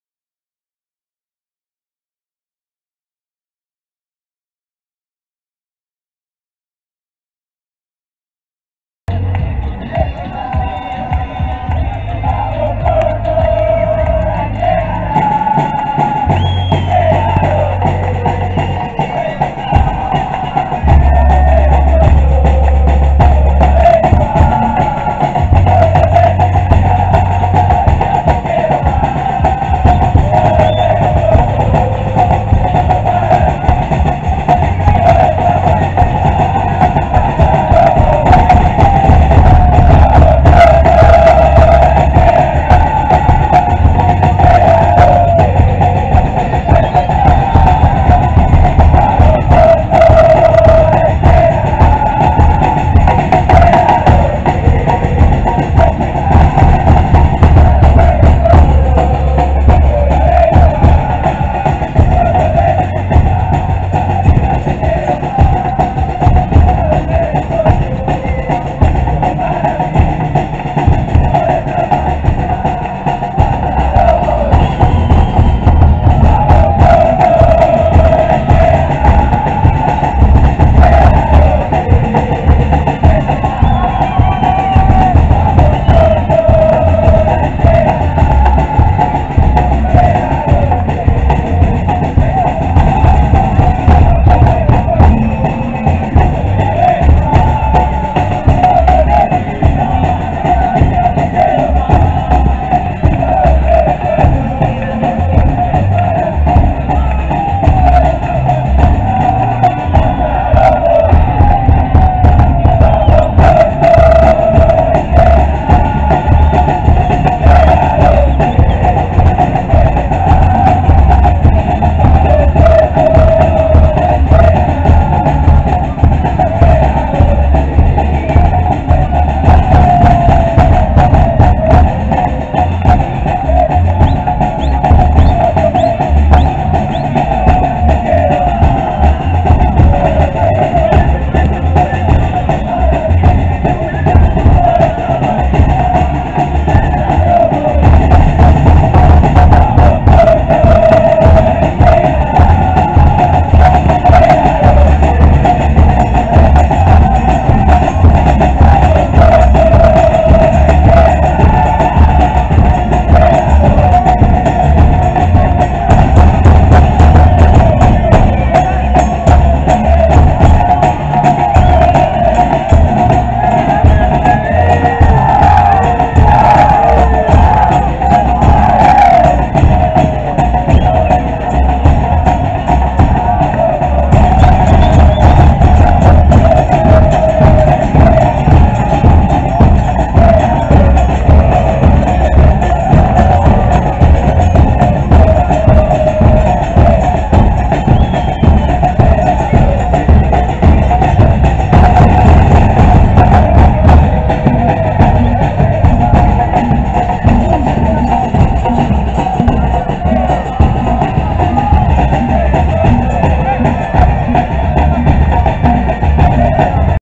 Aqui Esta La Mas Fiel La 12 Del Condor
Club Deportivo Condor De Pichidegua
La mejor barra que e visto en el futbol amateur, cantaron y gritaron cuando Iván ganando, empatando y perdiendo
Nacional Campeonato De Campeones Zona Norte 2020